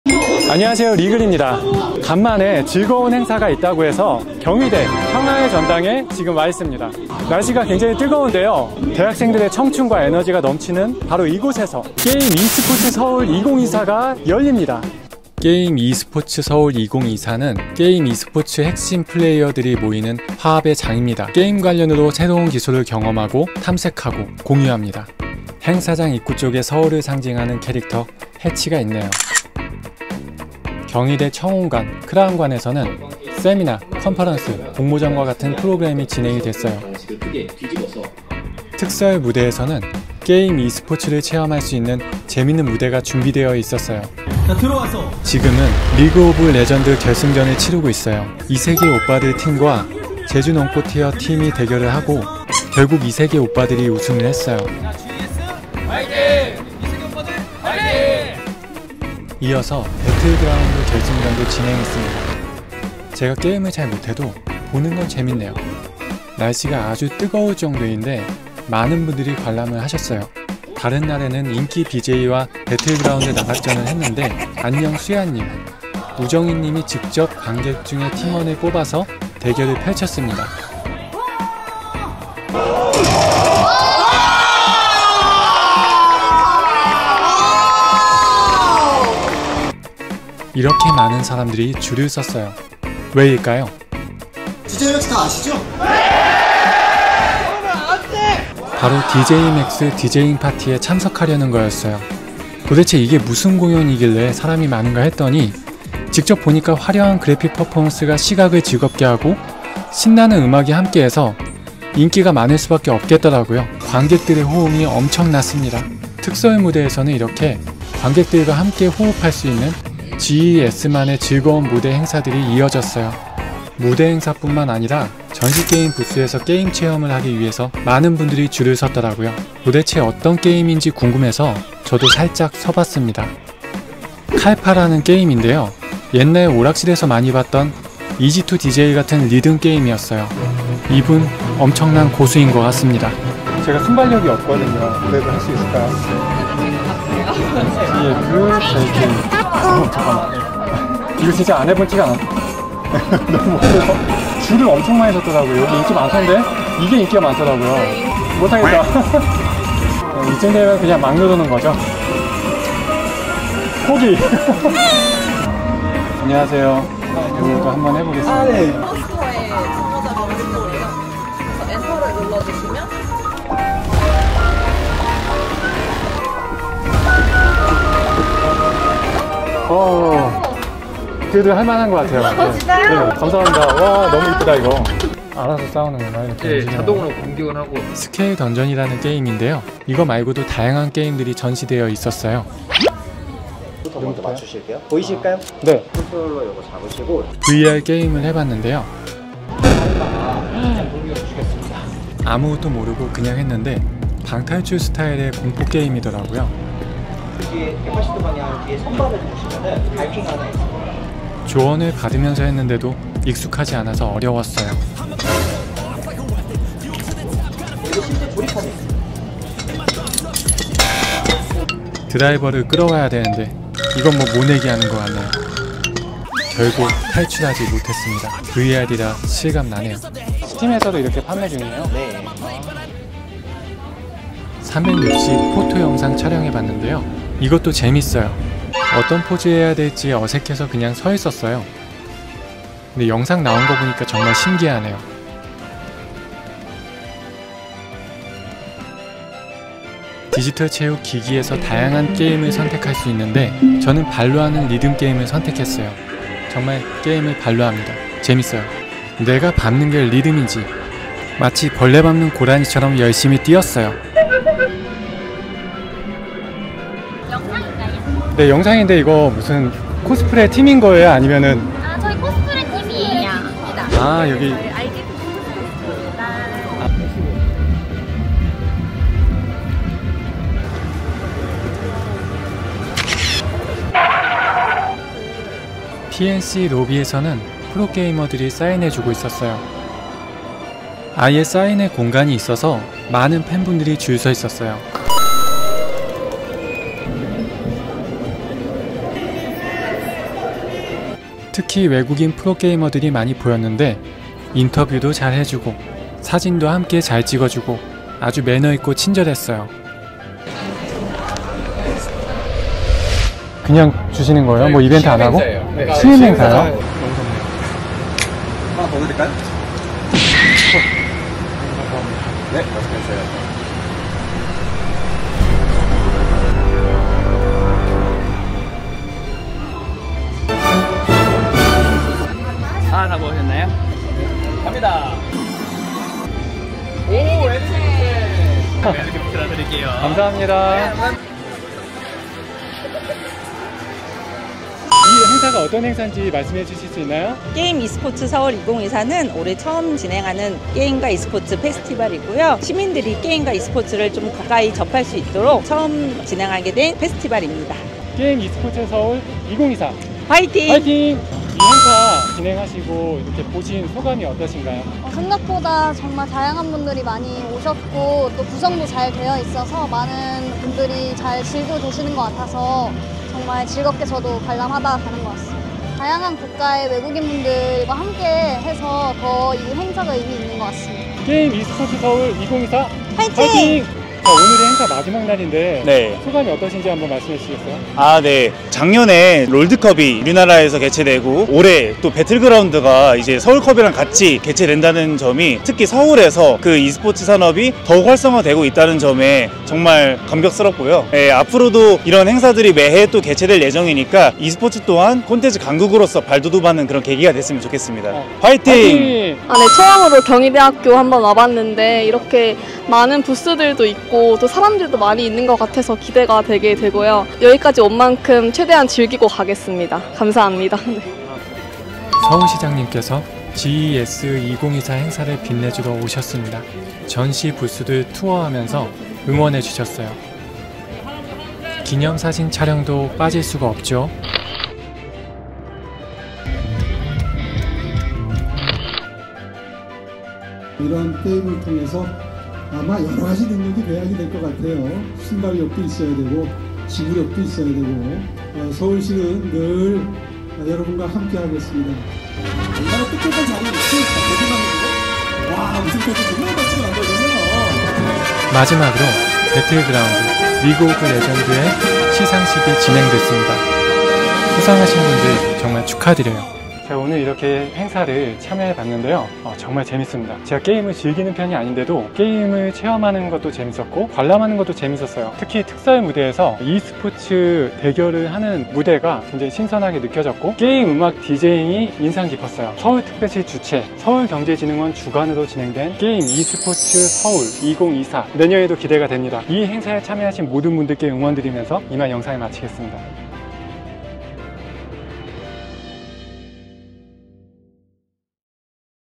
우정잉도 보고 실감나는 공포 VR게임도 하고 ㅣ Game Esports Seoul 2024 (게임 이스포츠 서울 GES2024)
게임이스포츠서울 2024 행사에 다녀왔습니다
리그오브레전드, 배틀그라운드 결승전을 무대에서
E스포츠 게임 대결을 관람하는 재미도 있었고,
전시게임 부스에서 다양한 게임을 직접 체험하며 즐겼습니다.
공포 VR게임 체험도 하고 인기BJ 안녕수야님, 우정잉님도 봤어요~

[촬영 제작 : 달빛] [편집 : 애린]